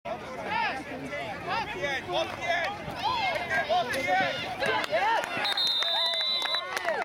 Issac untouched for TD
Good line blocking and Michea and Bruce the Wham!